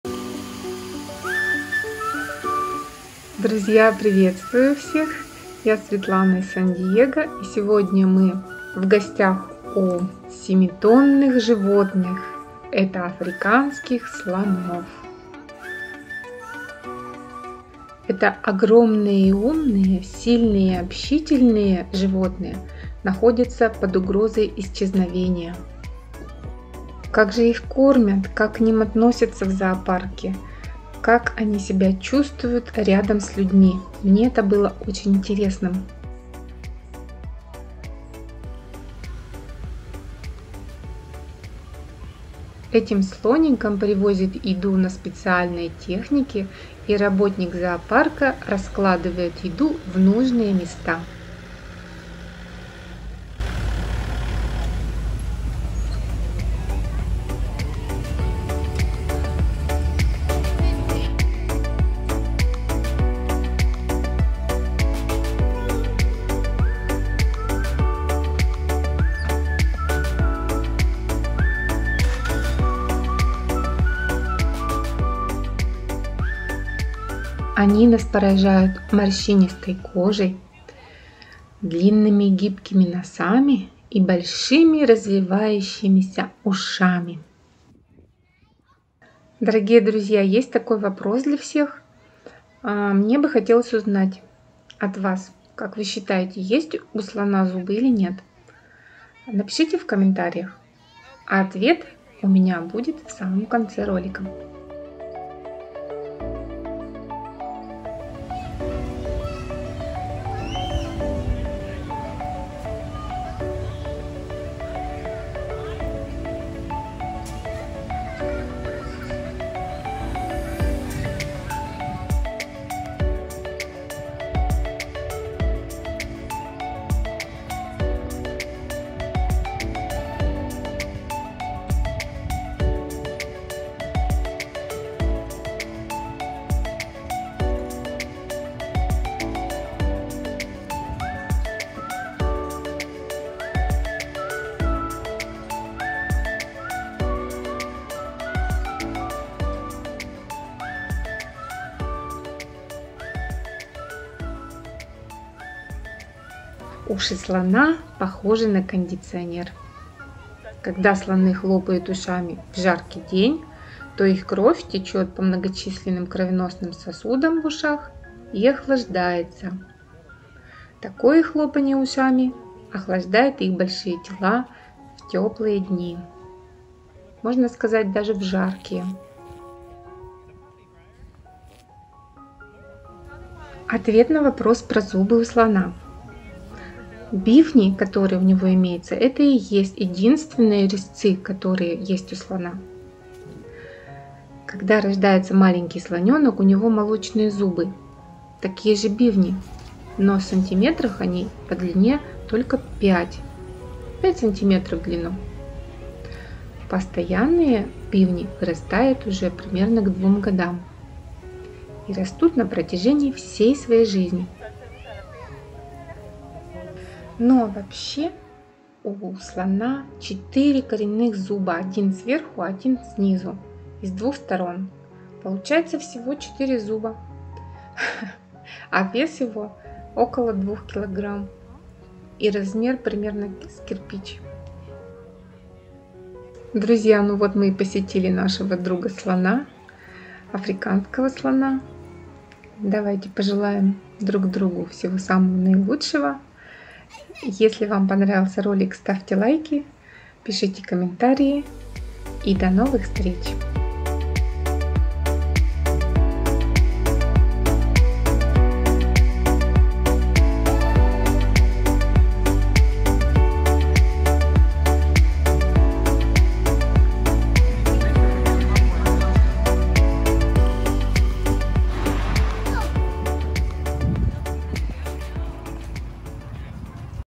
Elephants. Слоны. Мир животных в зоопарке Сан Диего.
Слоны Loxodonta africana (африканский).
Они огромные и умные, сильные и общительные. На протяжении веков люди восхищались слонами просто потому, что они такие большие - африканский слон-самец может весить до 7,5 тонн (6,8 метрических тонн)! Еще они поражают нас своими длинными и гибкими носами, большими развевающимися ушами и дряблой морщинистой кожей.
Уши слона похожи на кондиционер. Когда слоны хлопают ушами в жаркий день, кровь, текущая по многочисленным кровеносным сосудам в ушах, охлаждается. Если они только что плескались в реке, тем лучше! Такое хлопанье ушами охлаждает их большие тела в теплые дни.
Кожа слона на некоторых частях тела может достигать 1 дюйма (2,54 сантиметра). Кроме того, он свободный, из-за чего слон выглядит так, будто на нем мешковатые штаны или провисшие чулки. Но для этого есть веская причина - он держит слона в прохладе, задерживая влагу, которая дольше испаряется. Кожа слона хоть и толстая, но очень чувствительна к прикосновениям и солнечным ожогам. Слоны часто обрызгивают себя водой или катаются в грязи или пыли для защиты от солнца и укусов насекомых.
Бивни - это резцы слона и единственные резцы, которые есть у слона. Они используются для защиты, рытья в поисках воды и пищи и подъема вещей. Присутствующие при рождении бивни - это молочные зубы, которые выпадают через год, когда они достигают 5 сантиметров в длину. Постоянные бивни выходят за пределы губ примерно к двум-трем годам и растут на протяжении всей жизни слона.

Бивни состоят из слоновой кости (дентина) под внешним слоем эмали, но своеобразный ромбовидный узор бивня слона придает ему характерный блеск, которого не имеют бивни из слоновой кости других млекопитающих, таких как бегемоты, бородавочники, моржи и кашалоты. Браконьеры иногда убивают африканских слонов только из-за их клыков из слоновой кости.

У слонов также есть четыре коренных зуба, один сверху и один снизу с обеих сторон рта. Один коренной зуб может весить около 5 фунтов (2,3 кг) и равен размеру кирпича! Каждый слон за свою жизнь может пройти до шести наборов коренных зубов.

Новые зубы не прорезываются вертикально, как у большинства млекопитающих, а растут сзади, выталкивая старые изношенные зубы вперед и наружу, как производственная линия зубов, движущаяся вдоль челюсти сзади вперед. У очень старых слонов последний (шестой) набор коренных зубов может стать чувствительным и изношенным, и слоны предпочитают более мягкую пищу. Болота - идеальное место для мягкой растительной пищи, поэтому здесь часто встречаются старые слоны. Много раз они остаются там, пока не умрут. Эта практика натолкнула некоторых людей на мысль, что слоны уходили на специальные могильники умирать.
Хобот слона - это и верхняя губа, и нос. На каждой стороне туловища по 8 основных мышц и 150 000 мышечных пучков (частей мышц) на всем туловище. В этом уникальном придатке нет костей и хрящей. Хобот слона настолько силен, что может сбивать деревья, и настолько проворен, что может подобрать один кусок соломы. Слоны также используют свой хобот, как и мы руками: чтобы хватать, удерживать, поднимать, дотягиваться, касаться, тянуть, толкать и бросать.

Туловище по-прежнему представляет собой нос, на конце которого есть две ноздри, которые всасывают воздух через длинные носовые ходы в легкие. Слоны тоже пьют хоботом, но вода не доходит до носа, как соломинка; вместо этого слон сосет воду только частично вверх по хоботу, заворачивает ее ко рту, поднимает голову вверх и позволяет воде из хобота вливаться внутрь.
Слоны издают много разных звуков; люди не могут слышать некоторые из этих звуков, поскольку их частота слишком мала для нашего уха. Слоны используют эти звуки для общения друг с другом на больших расстояниях. У вас когда-нибудь урчал живот в неудачный момент? Что ж, рычание в животе - долгожданный звук в слоновьем обществе; живот, издающий громкое урчание и рычание, кажется, сигнализирует другим, что все в порядке.
Слоны в зоопарке Сан-Диего и сафари-парке зоопарка Сан-Диего едят немного меньше, чем их собратья в саванне - около 125 фунтов (57 килограммов) еды каждый день - потому что им не нужно сжигать столько калорий, сколько нужно. еда. Тем не менее, взрослым самцам африканских слонов в Сафари-парке необходимо съедать более 70 000 калорий каждый день! Нашим слонам ежедневно предлагают сено, гранулы травоядных животных, акацию, сельдерей, огурцы и салат. Слоны пьют от 20 до 50 галлонов (от 75 до 190 литров) воды каждый день.